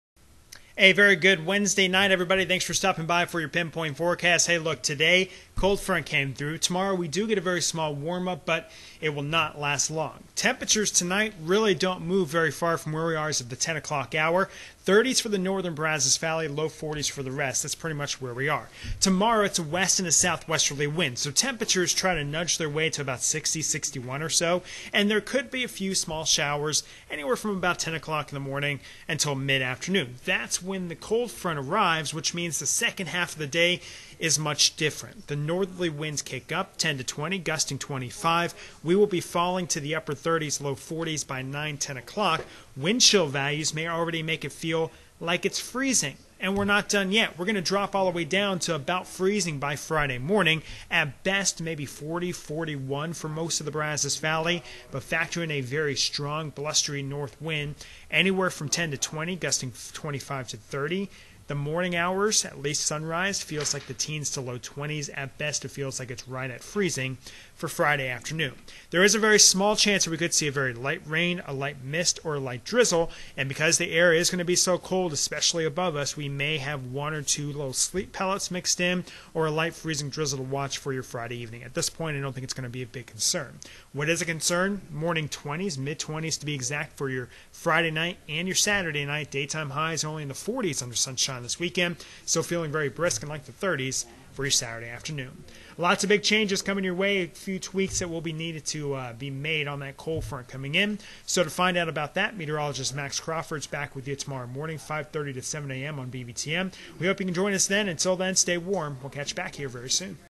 Wednesday Night PinPoint Weather Update 1.4.2017
Wednesday Night PinPoint Weather Update with Chief Meteorologist Shel Winkley